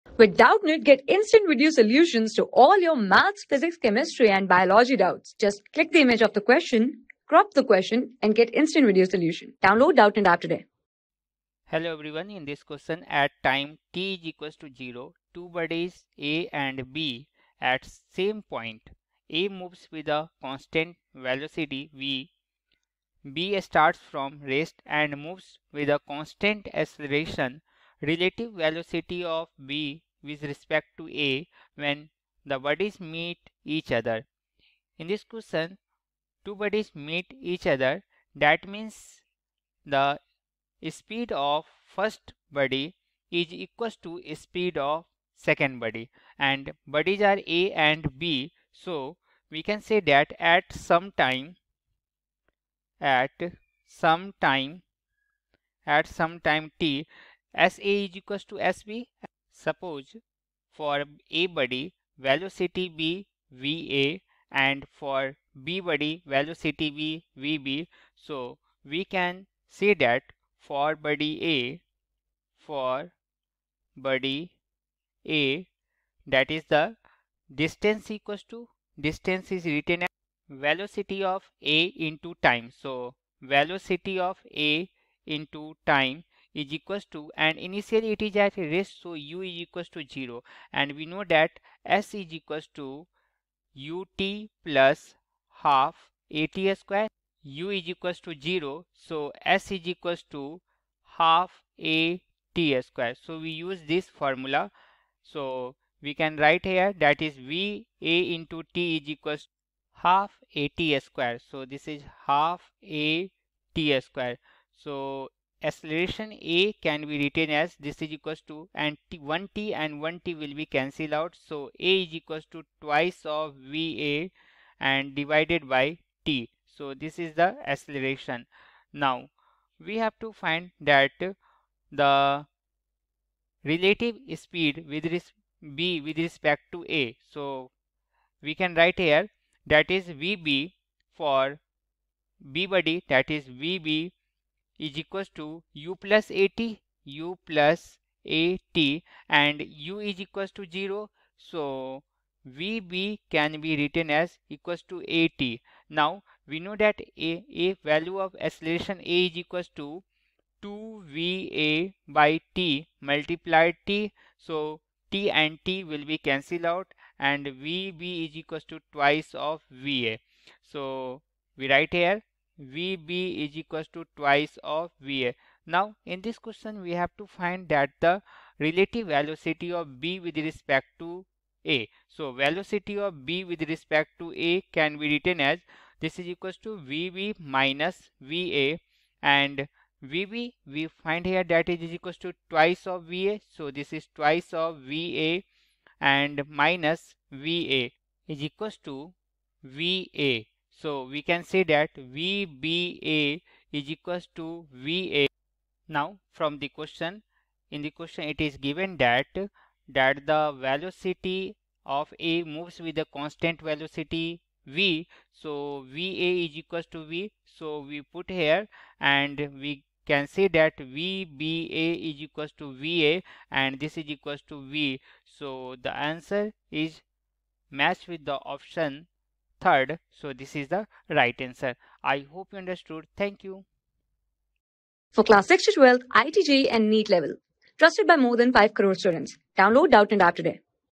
At time t = 0, two bodies A and B at the same point. A moves with constant velocity upsilon and ...
At time t = 0, two bodies A and B at the same point. A moves with constant velocity upsilon and B starts from rest and moves with constant acceleration. Relative velocity of B w.r.t. A when the bodies meet each other is
Class: 11
Subject: PHYSICS
Chapter: MOTION IN A PLANE
Board:IIT JEE

👉 Have Any Query? Ask Us.